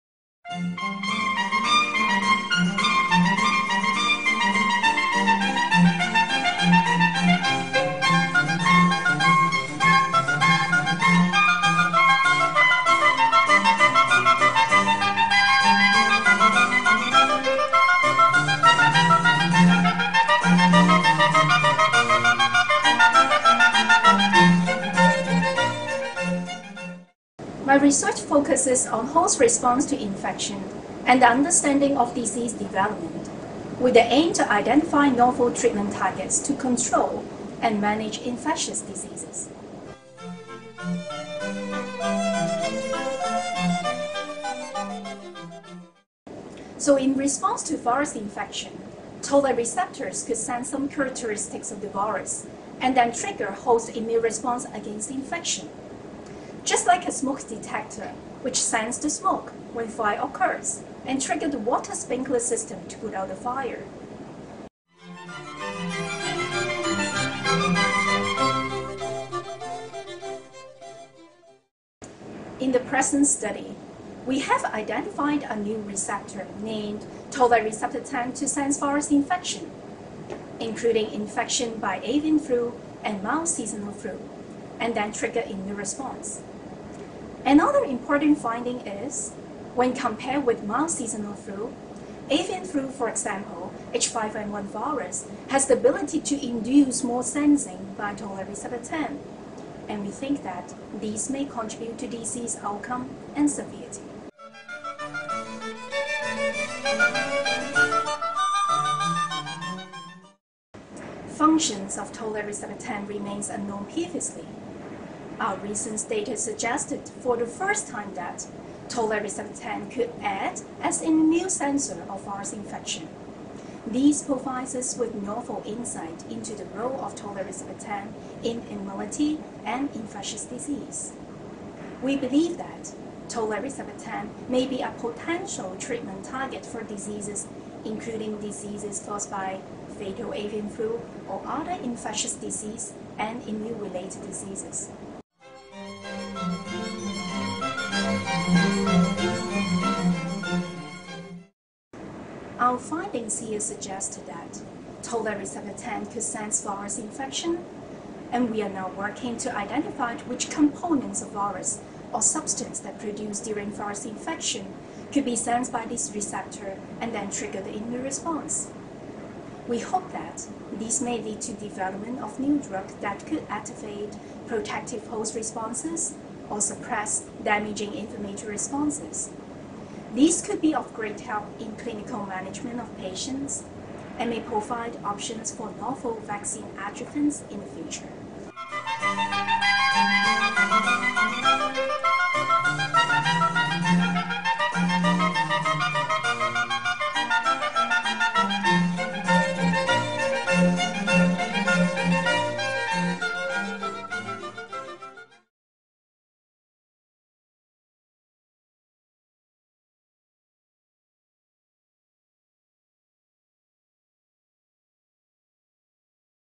Featured Publication: Dr Lee Man Yan
Lee M.Y., Kok K.H., Jaume M.D.A., et al., Toll-like receptor 10 is involved in induction of innate immune responses to influenza virus infection, Proceedings of the National Academy of Sciences of the United States of America. 2014, 111(10): 3793-8.